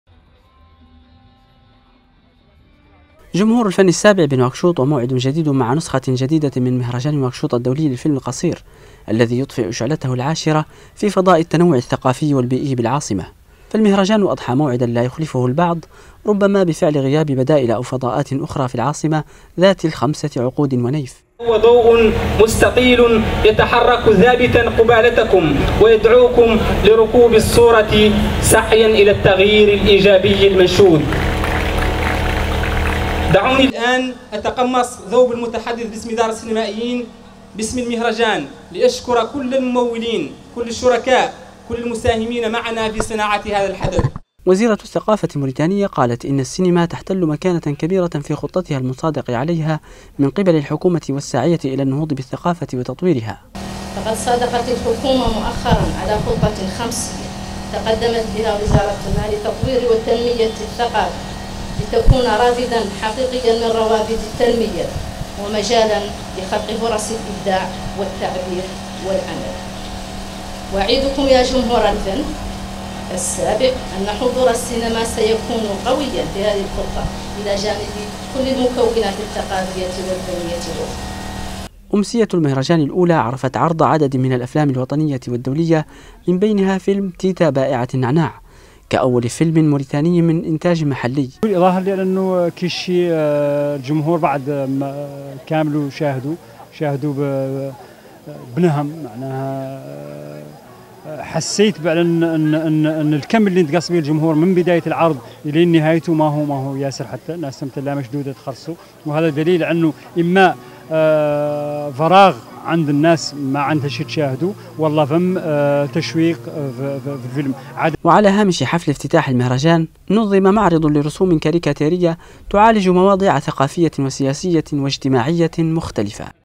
انطلاق النسخة العاشرة من مهرجان نواكشوط الدولي للفيلم القصير تقرير المختار ولد محمد امبارك.
انطلقت بالعاصمة نواكشوط النسخة العاشرة من مهرجان نواكشوط الدولي للفيلم القصير، الذي تنظمه دار السينمائيين الموريتانيين.
هذا وستتضمن نسخة هذا العام خمسين فيلما قصيرا من بينها مشاركات من ثماني دول، كما ستنظم على هامشه ست دورات تكوين وثلاث ندوات.
تقرير: المختار ولد محمد امبارك.